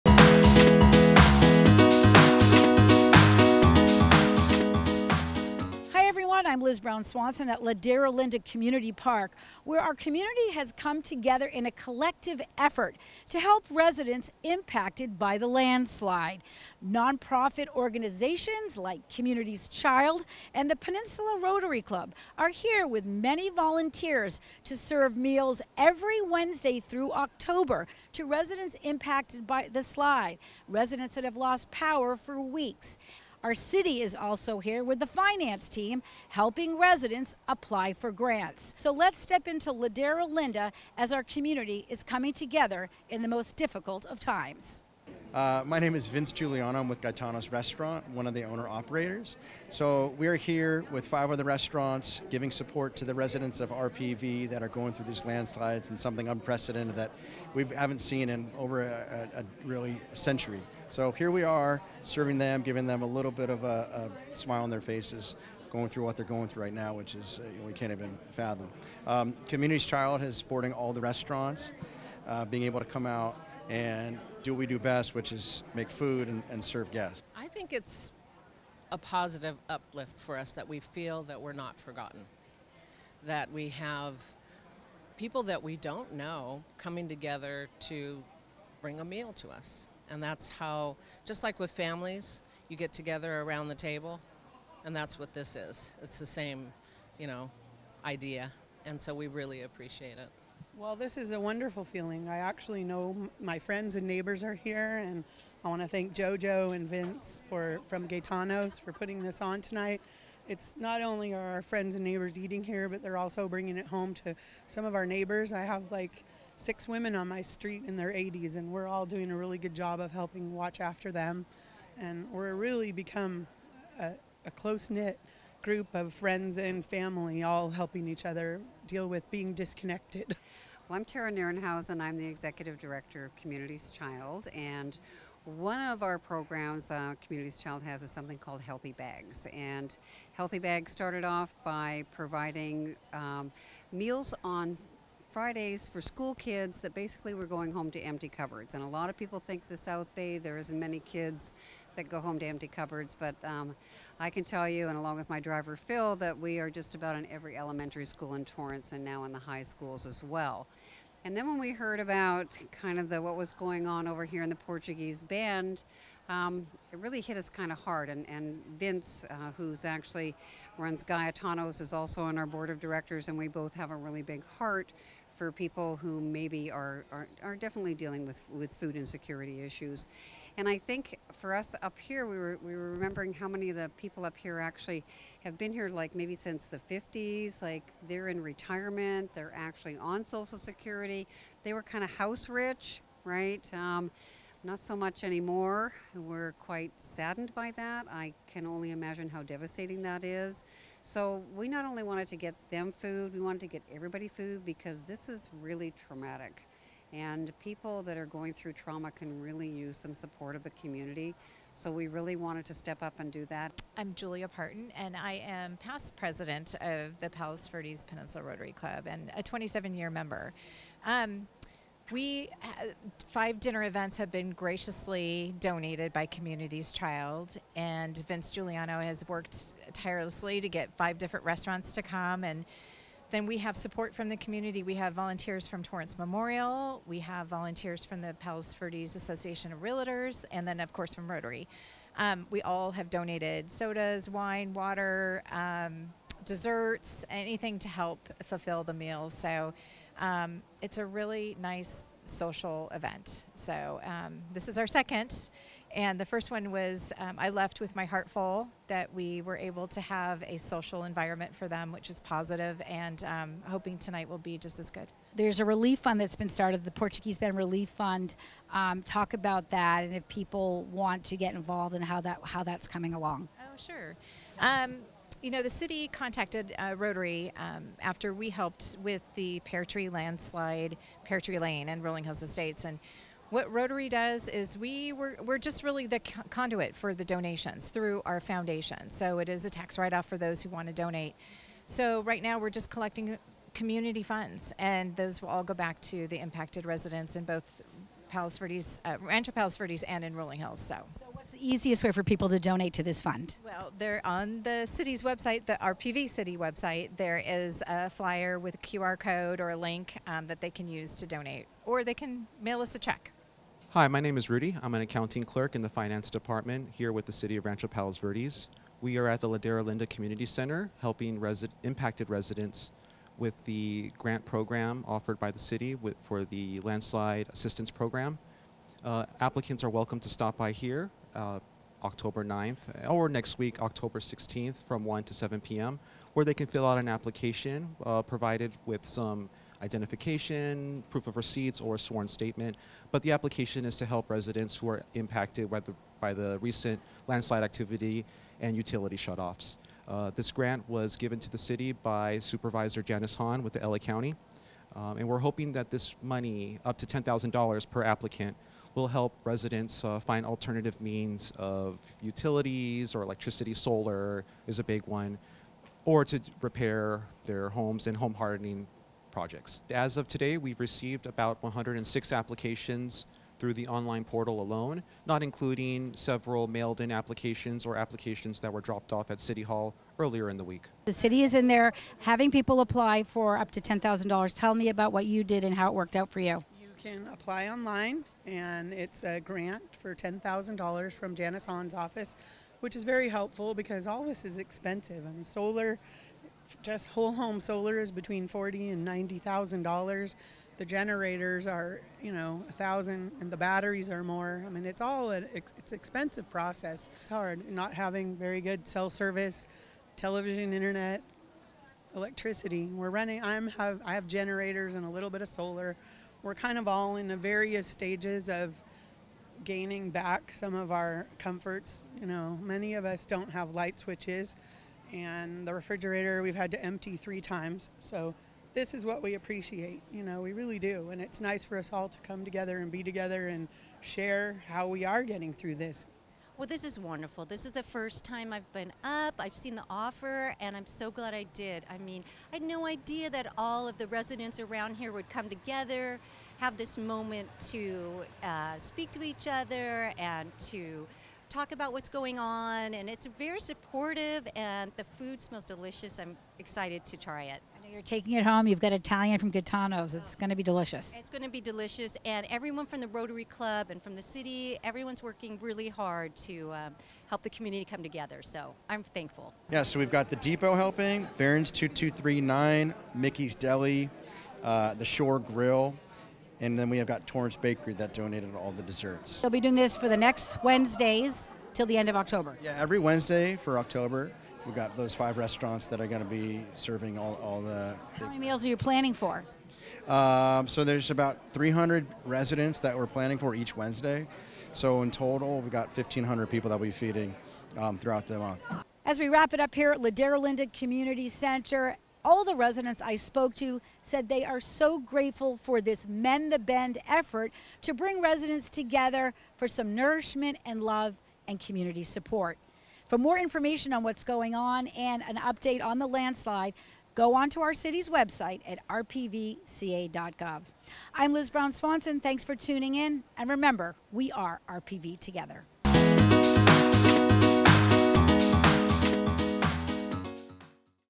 RPVtv Update - "Mend The Bend"
RPVtv's Liz Brown Swanson reports from Ladera Linda Community Center where the community and city officials  come together in a collective effort to support residents impacted by the ongoing landslide crisis in Rancho Palos Verdes, CA.